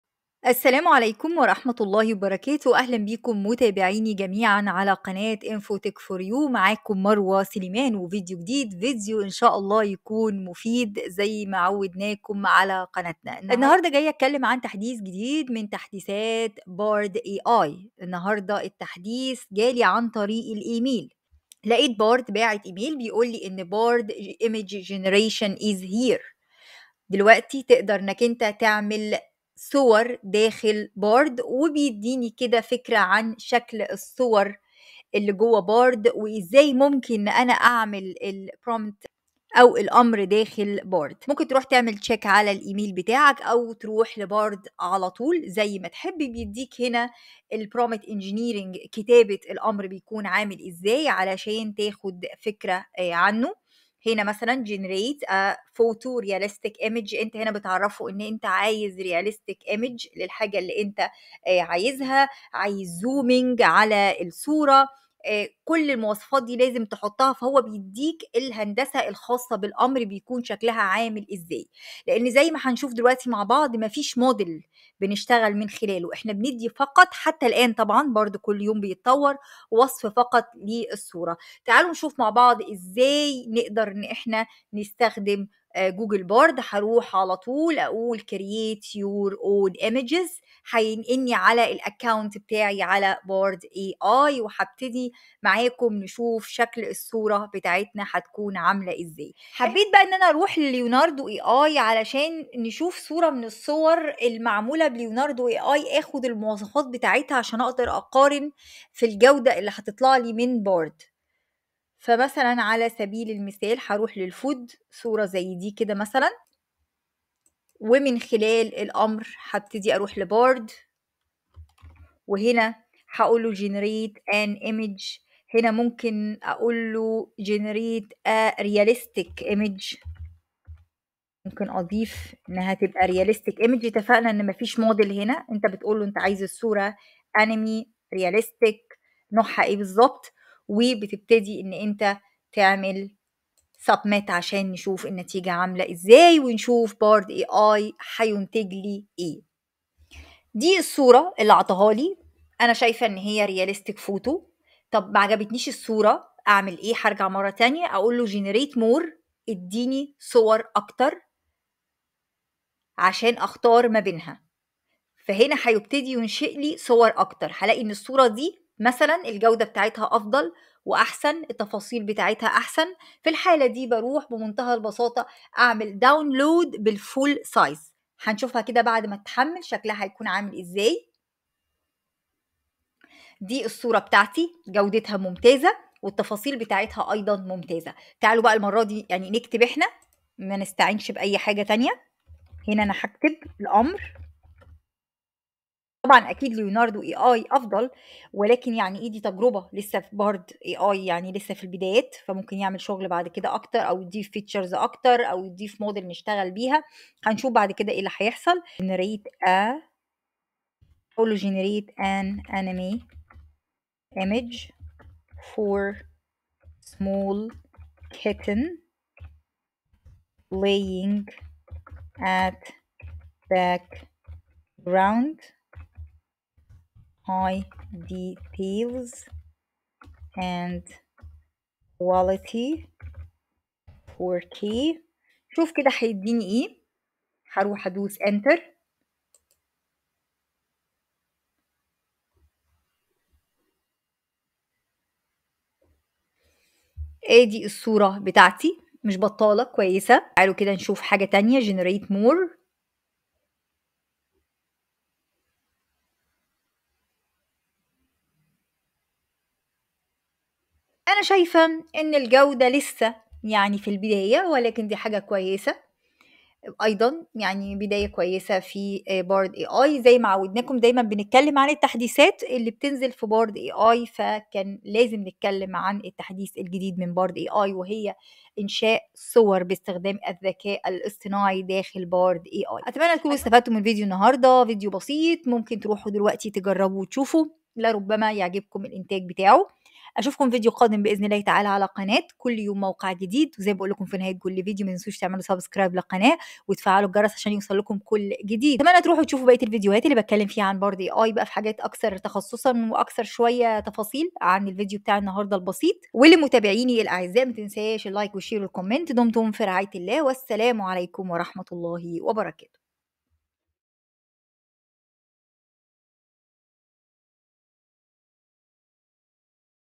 توليد الصور بالذكاء الاصطناعي | تحديث جديد | Bard AI
الآن التحديث الجديد من بارد bard وهو توليد الصور بالذكاء الاصطناعي